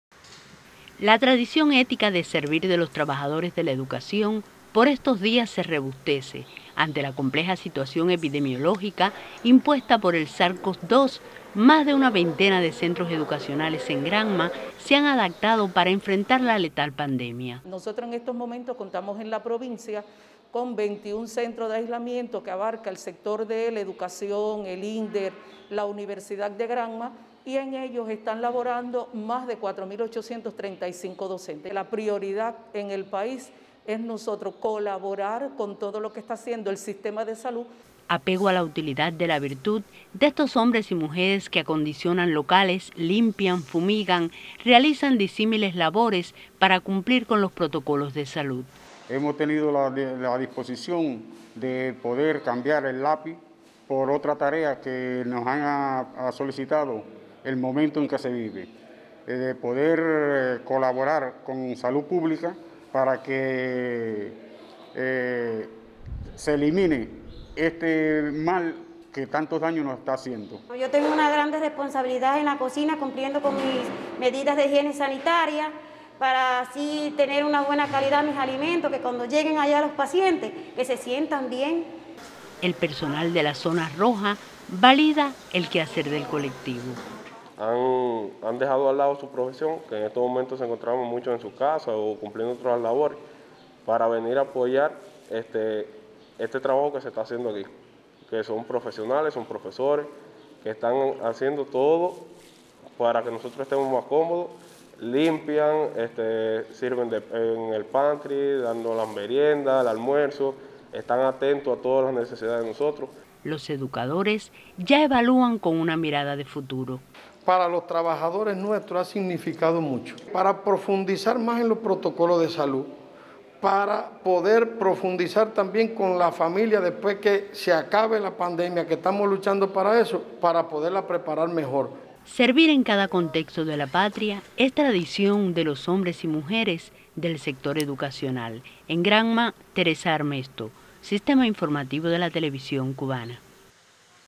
Laboran trabajadores de la educación en centros de aislamiento en el enfrentamiento a la COVID 19
En  Granma ante la compleja situación  epidemiológica, se han creado   56 centros de aislamiento, más de una veintena de ellos,  son instituciones  educacionales, la  escuela especial  Félix Varela de la capital granmense es representativa de los ajustes para  prestar  servicios a pacientes sospechosos  ante las urgencias  impuestas por la COVID  19 .